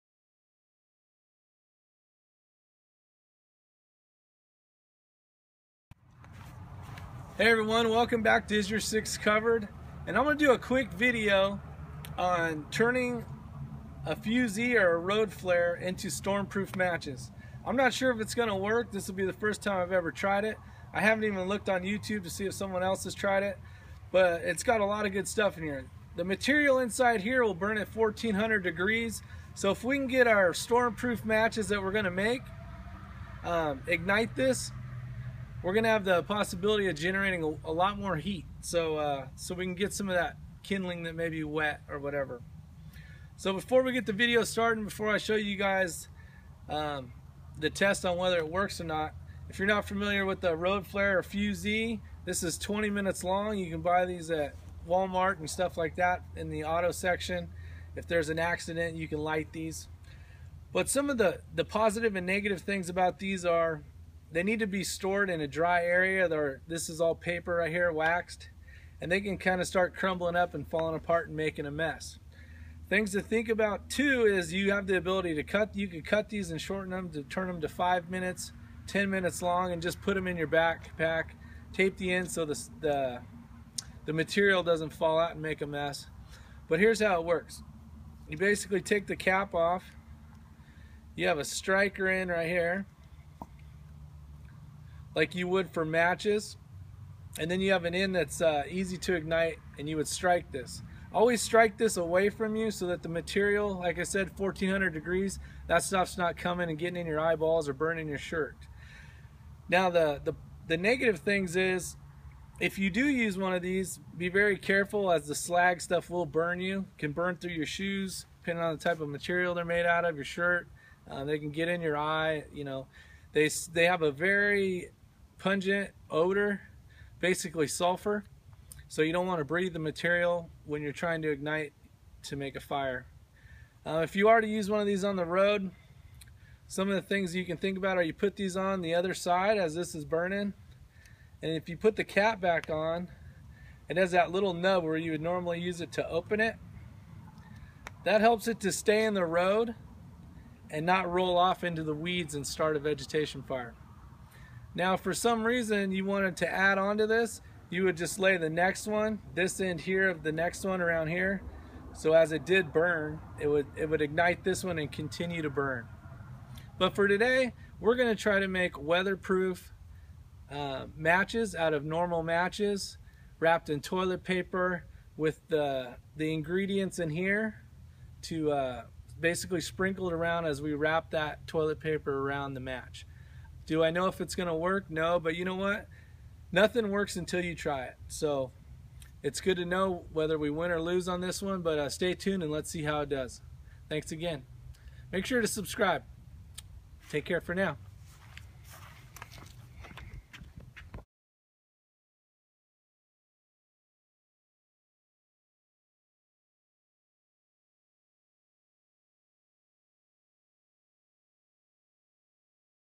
BEST DIY Storm Proof Matches made from road flares Part 1
I will show you how to make the best storm proof matches. These are the greatest matches made with road flare material, toilet paper and wax. Tell me what you think. I bet you make some if you need them.